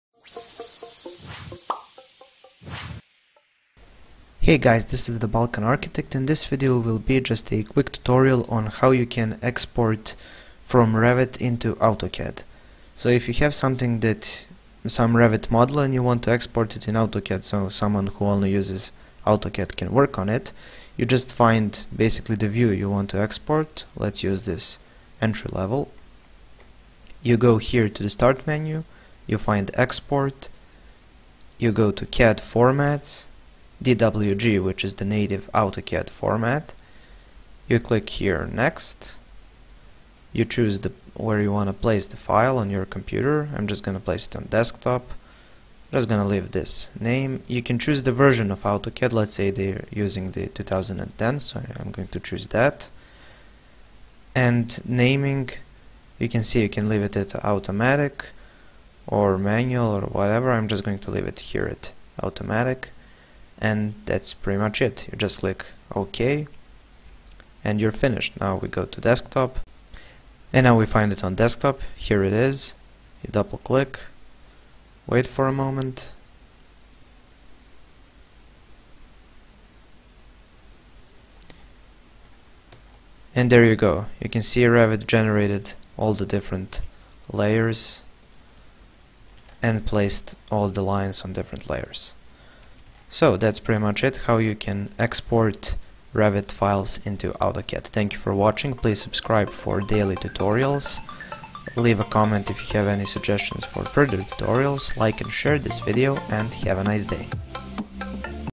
Exporting Revit to AutoCAD / DWG Tutorial
Learn how to export from Revit to AutoCAD DWG files Tutorial!